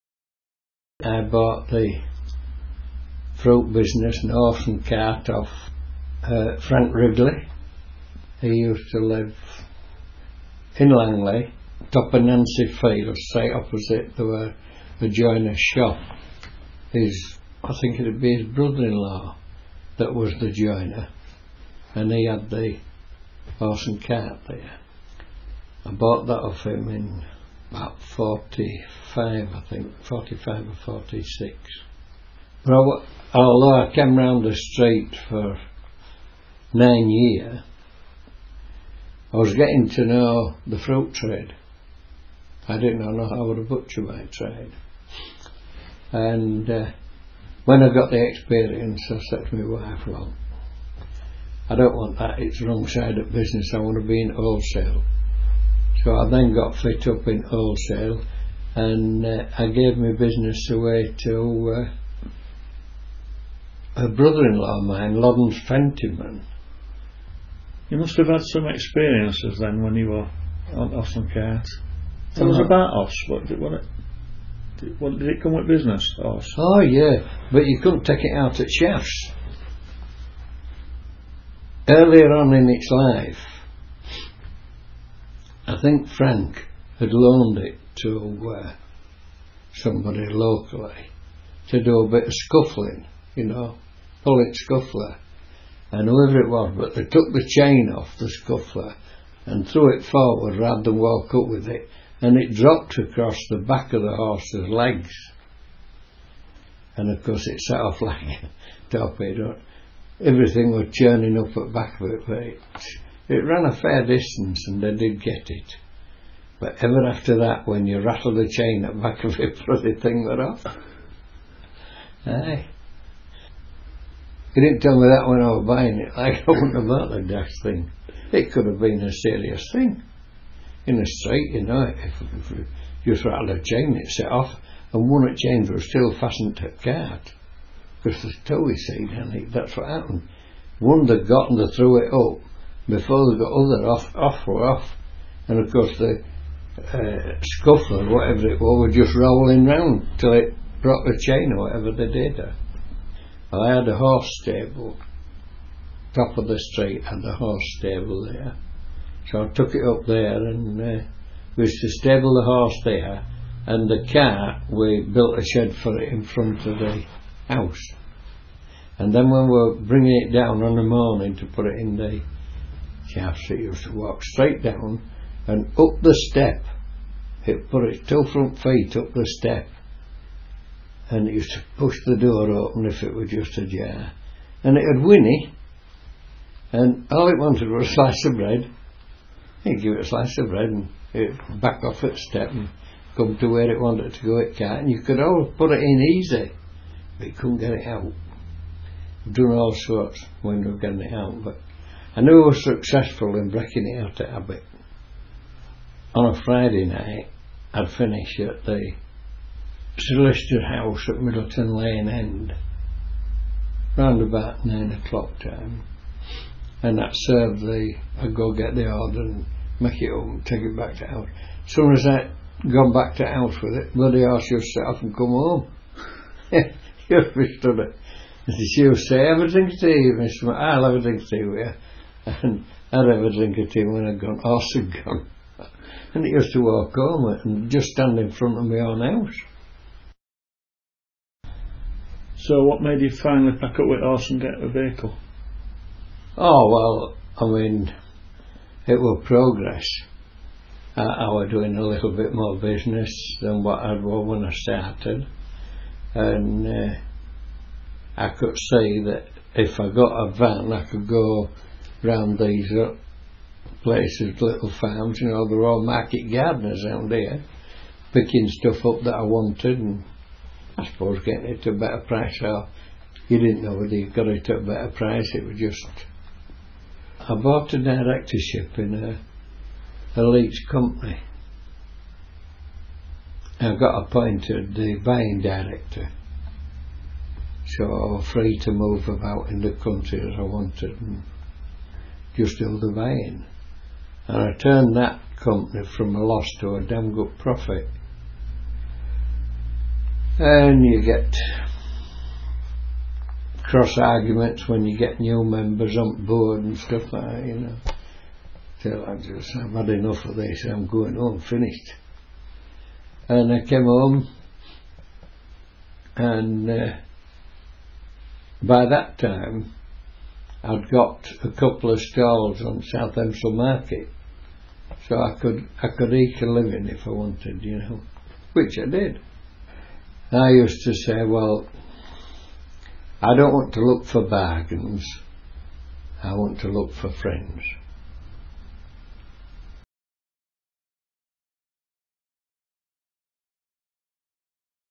A Hawkers Tales
Every village had it's Hawker, here we listen to Bernard Marshall from Lofthouse, the photo is of Herbert Broad from Outwood.